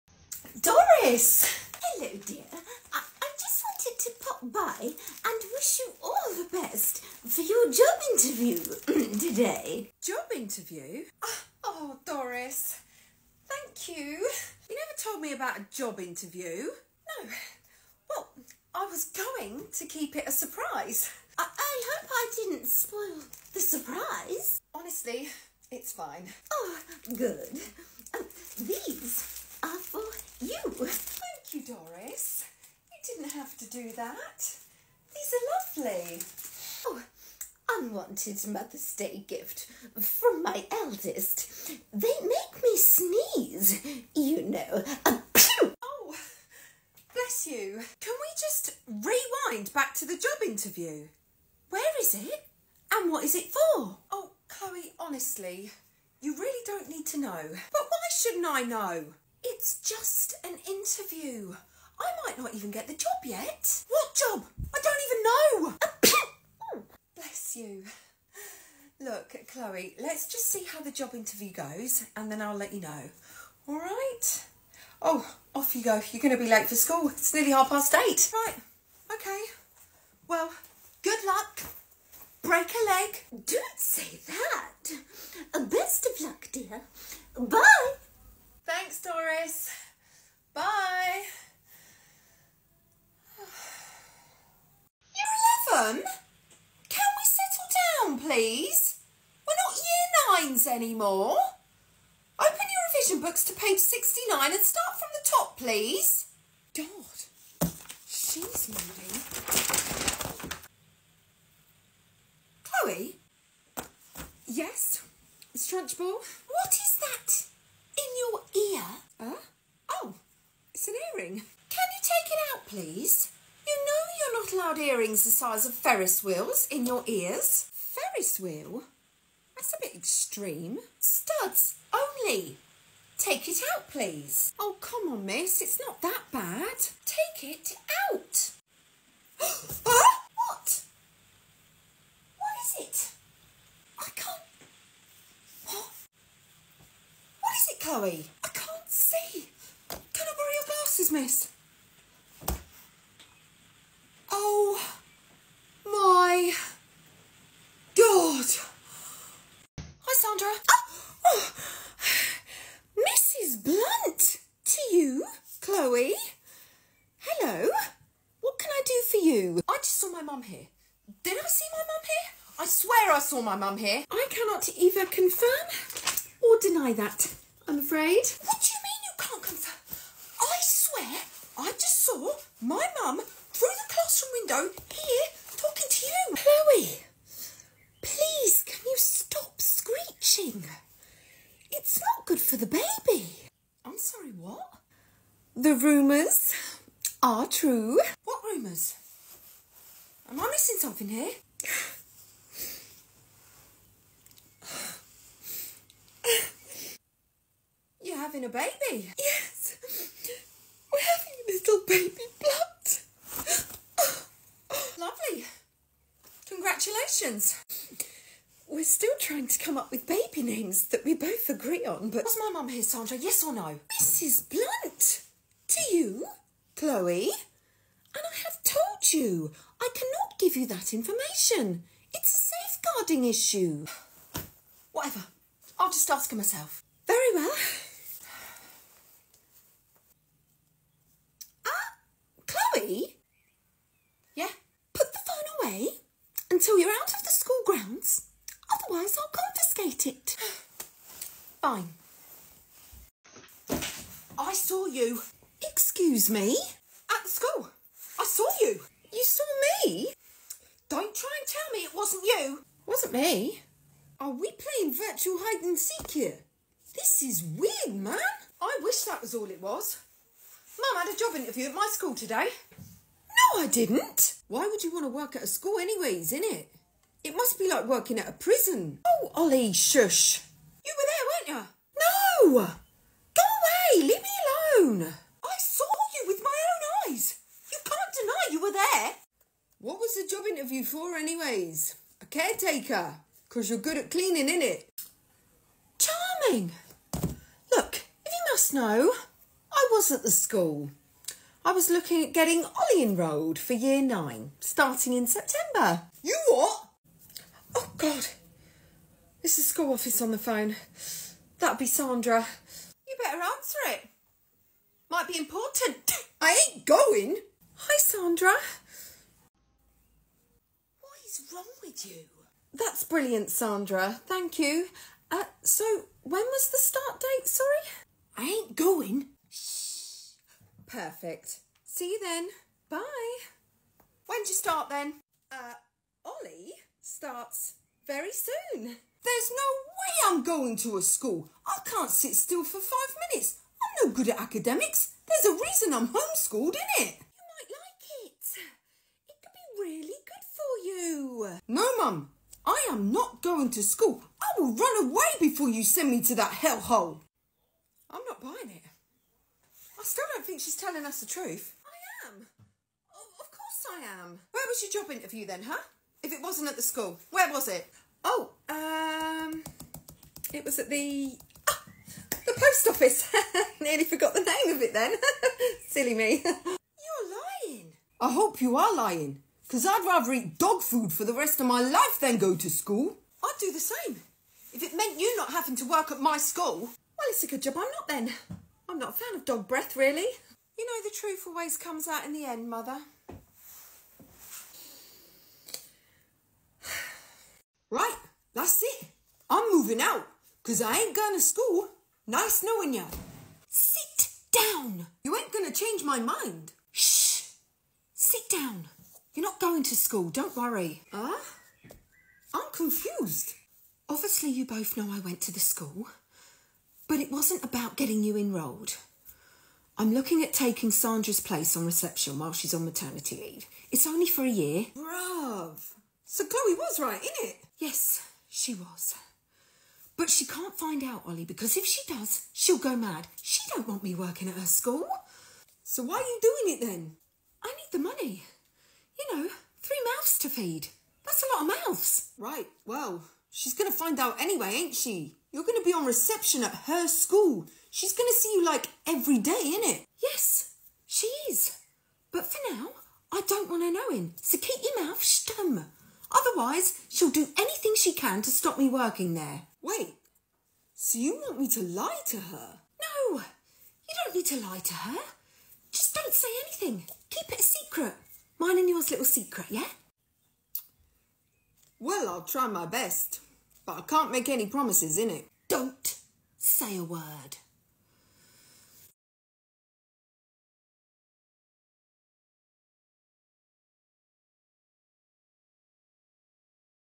Teen Daughter Uncovers Mums HUGE Secret!
🤫📚 Brace yourself for a laugh-out-loud comedy sketch that will leave you in stitches! Join us as we unveil the uproarious tale of a mum with a secret so big, it's bound to shake up her daughter's world! In this side-splitting scenario, teenage daughter Chloe discovers that her mum has been keeping a hilarious secret – she's the new receptionist at her school! 😂🏫

Get ready to witness teenage drama at its finest as Chloe's world turns upside down with this unexpected revelation. From the initial shock to the ensuing chaos, our comedy sketch captures every moment of hilarity as Chloe grapples with the outrageous situation her mum has landed them in.

Watch as the tension mounts and the comedy escalates with each awkward encounter between mother and daughter in the school corridors. With snappy one-liners, priceless facial expressions, and a dash of teenage angst, this sketch promises non-stop laughter from start to finish!

Join us on this riotous journey as we navigate through the absurdity of family secrets, teenage melodrama, and the ultimate test of mother-daughter relationships. Whether you're a parent, a teenager, or just a fan of comedy gold, this video is guaranteed to tickle your funny bone and leave you craving for more!